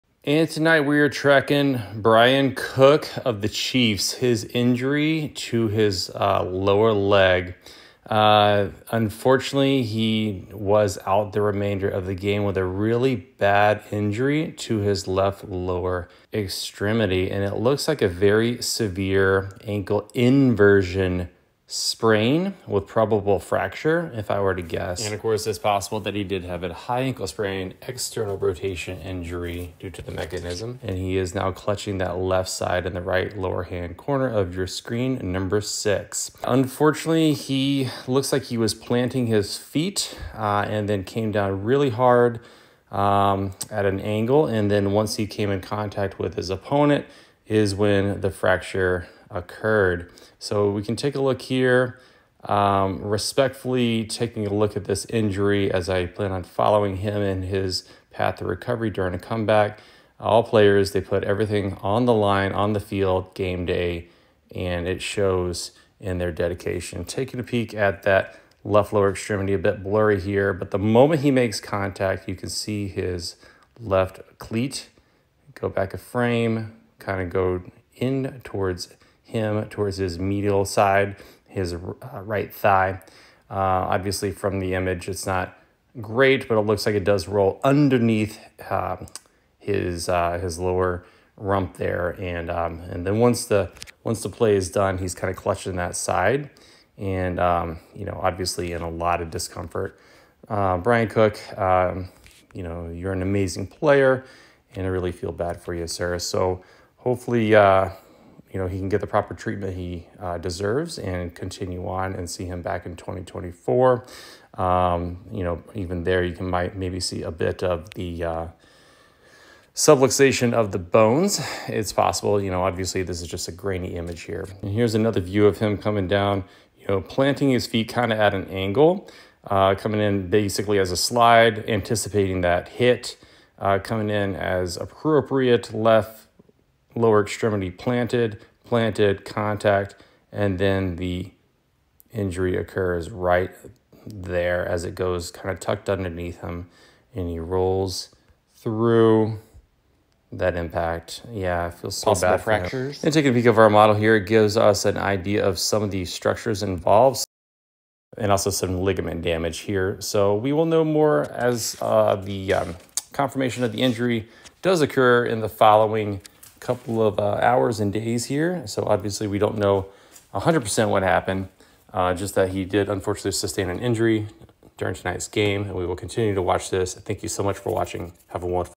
Bryan Cook Carted off field after HORRIBLE Ankle Injury
In a solemn moment on the field, Bryan Cook faced a distressing lower leg injury, leading to his cart-off. Our thoughts and best wishes go out to him during this challenging time. In this video, we approach the incident with the utmost respect, providing an account of the unfortunate event while maintaining sensitivity to the player's well-being. Join us in sending positive vibes and hoping for a swift recovery for Bryan Cook. 🙏🏈